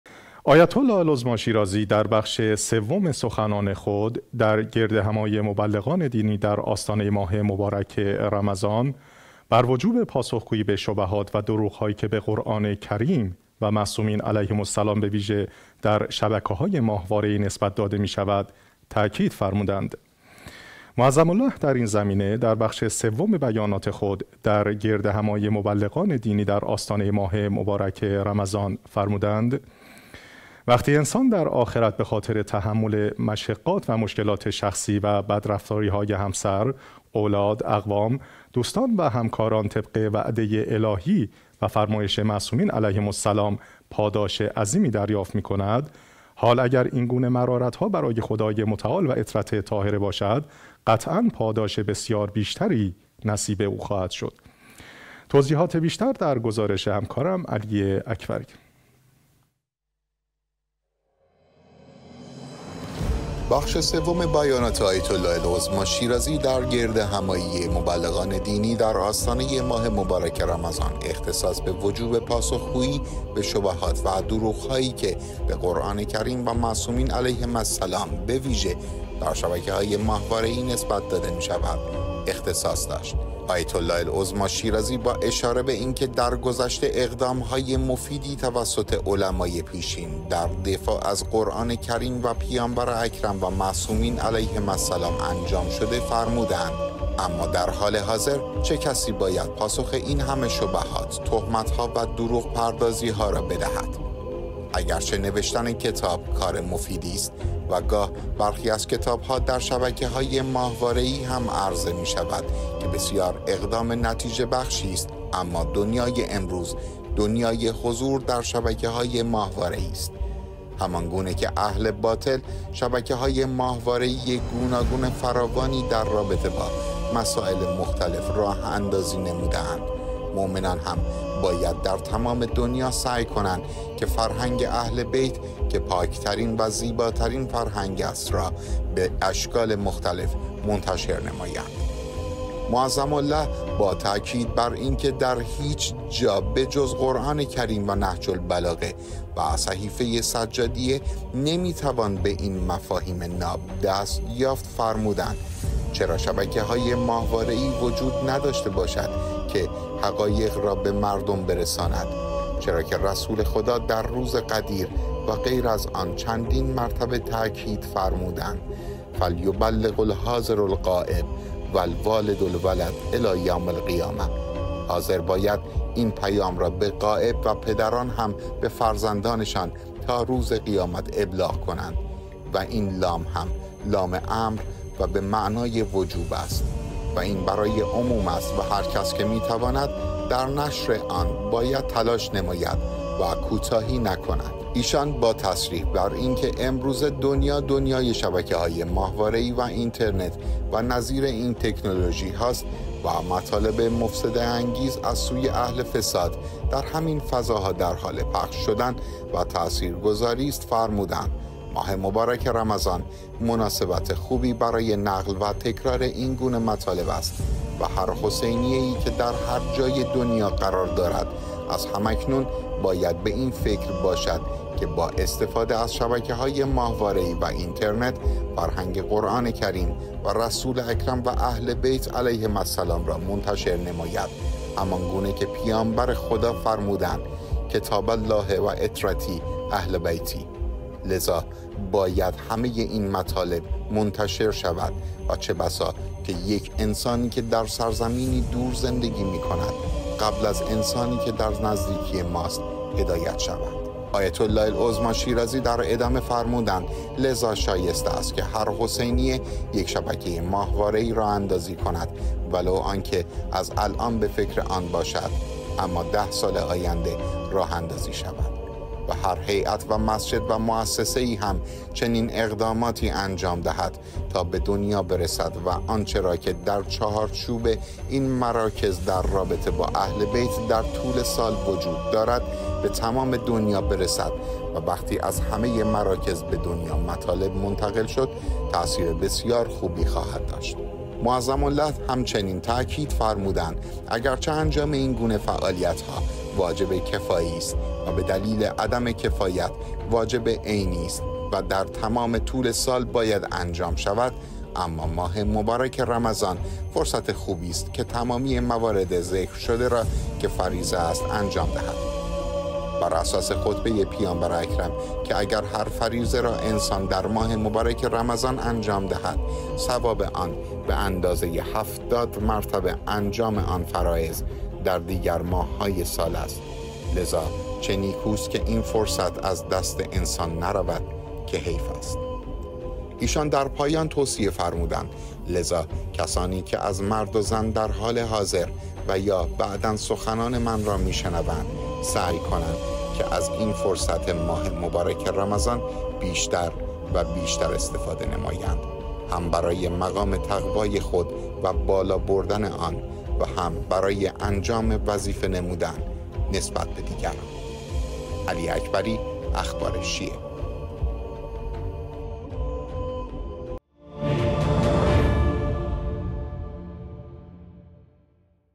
توصیه آیت الله العظمی شیرازی بر وجوب پاسخگویی به شبهاتی که به قرآن کریم و معصومین نسبت داده می شود
👈 توضیحات بیشتر در گزارش  علی اکبری
آیت الله العظمی شیرازی در بخش سوم سخنان خود در گردهمایی مبلغان دینی در آستانه ماه مبارک رمضان بر وجوب پاسخگویی به شبهات و دروغ هایی که به قرآن کریم و معصومین علیهم السلام به ویژه در شبکه های ماهواره ای نسبت داده می شود،  تاکید فرمودند.
معظم له در این زمینه، در بخش سوم بیانات خود در گردهمایی مبلغان دینی در آستانه ی ماه مبارک رمضان فرمودند : وقتی انسان در آخرت به خاطر تحمل مشقات و مشکلات شخصی و بدرفتاری های همسر، اولاد، اقوام، دوستان و همکاران، طبق وعده الهی و فرمایش معصومین علیهم السلام، پاداش عظیمی دریافت می کند، حال اگر این گونه مرارت ها برای خدای متعال و عترت طاهره باشد قطعا پاداش بسیار بیشتری نصیب او خواهد شد.

بخش سوم بیانات آیت الله العظمی شیرازی در گردهمایی مبلغان دینی در آستانه ماه مبارک رمضان اختصاص به وجوب پاسخگویی به شبهات و دروغ هایی که به قرآن کریم و معصومین علیهم السلام به ویژه در شبکه های ماهواره ای نسبت داده می شود، اختصاص داشت.
آیت الله العظمی شیرازی با اشاره به اینکه در گذشته اقدام های مفیدی توسط علمای پیشین در دفاع از قرآن کریم و پیامبر اکرم و معصومین علیهم السلام انجام شده فرمودند: اما در حال حاضر چه کسی باید پاسخ این همه شبهات، تهمت ها و دروغ پردازی ها را بدهد؟ اگر چه نوشتن کتاب، کار مفیدی است و گاه برخی از کتاب ها در شبکه های ماهواره ای هم عرضه می شود که بسیار اقدام نتیجه بخشی است اما دنیای امروز، دنیای حضور در شبکه های ماهواره ای است، همان گونه که اهل باطل شبکه های ماهواره ای گوناگون فراوانی در رابطه با مساُئل مختلف راه اندازی نموده اند، مومنان هم باید در تمام دنیا سعی کنند که فرهنگ اهل بیت که پاک ترین و زیباترین فرهنگ است را به اشکال مختلف منتشر نمایند.
معظم له با تاکید بر اینکه در هیج جا به جز قرآن کریم و نهج البلاغه و صحیفه سجادیه نمی توان به این مفاهیم ناب دست یافت فرمودند : چرا شبکه های ماهواره ای وجود نداشته باشد که این حقایق را به مردم برساند، چرا که رسول خدا در روز غدیر و غیر از آن، چندین مرتبه تاکید فرمودند فَلْیُبَلِّغِ الْحاضِرُ الْغائِبَ وَالْوالِدُ الْوَلَدَ إِلی یَوْمِ الْقِیامَةِ، حاضر باید این پیام را به غائب و پدران هم به فرزندانشان، تا روز قیامت ابلاغ کنند، و این لام هم، لام امر و به معنای وجوب است و این برای عموم است و هر کس که می تواند، در نشر آن باید تلاش نماید و کوتاهی نکند.
ایشان با تصریح بر اینکه امروزه دنیا، دنیای شبکه های ماهواره ای و اینترنت و نظیر این تکنولوژی هاست و مطالب مفسده انگیز از سوی اهل فساد در همین فضاها در حال پخش شدن و تاثیرگذاری است فرمودند : ماه مبارک رمضان، مناسبت خوبی برای نقل و تکرار این گونه مطالب است و هر حسینیه ای که در هر جای دنیا قرار دارد، از هم اکنون باید به این فکر باشد که با استفاده از شبکه های ماهواره ای و اینترنت، فرهنگ قرآن کریم و رسول اکرم و اهل بیت علیهم السلام را منتشر نماید همانگونه که پیامبر خدا فرمودند: كِتَابَ اللهِ وَ عِتْرَتِی أَهْلَ بَیتِی، لذا باید همه ی این مطالب منتشر شود و چه بسا که یک انسانی که در سرزمینی دور زندگی می کند قبل از انسانی که در نزدیکی ماست هدایت شود.
آیت الله العظمی شیرازی در ادامه فرمودند: لذا شایسته است که هر حسینیه یک شبکه ماهواره ای راه اندازی کند ولو آنکه از الان به فکر آن باشد اما ده سال آینده راه اندازی شود، و هر هیات و مسجد و موسسه ای هم، چنین اقداماتی انجام دهند تا به دنیا برسد و آنچه را که در چارچوب این مراکز در رابطه با اهل بیت در طول سال وجود دارد به تمام دنیا برسد و وقتی از همه مراکز به دنیا مطالب منتقل شد تاثیر بسیار خوبی خواهد داشت.
معظم له هم چنین تاکید فرمودند: اگر چه انجام این گونه فعالیت ها واجب کفایی است و به دلیل عدم کفایت، واجب عینی است و در تمام طول سال باید انجام شود اما ماه مبارک رمضان فرصت خوبی است که تمامی موارد ذکر شده را که فریضه است انجام دهد، بر اساس خطبه ی پیامبر اکرم که اگر هر فریضه را انسان در ماه مبارک رمضان انجام دهد، ثواب آن به اندازه ی هفتاد مرتبه انجام آن فرائض، در دیگر ماه های سال است لذا چه نیکوست که این فرصت از دست انسان نرود که حیف است.
ایشان در پایان توصیه فرمودند : لذا کسانی که از مرد و زن در حال حاضر و یا بعدا سخنان من را می شنوند، سعی کنند که از این فرصت ماه مبارک رمضان بیشتر و بیشتر استفاده نمایند هم برای مقام تقوای خود و بالا بردن آن و هم برای انجام وظیفه نمودن نسبت به دیگران.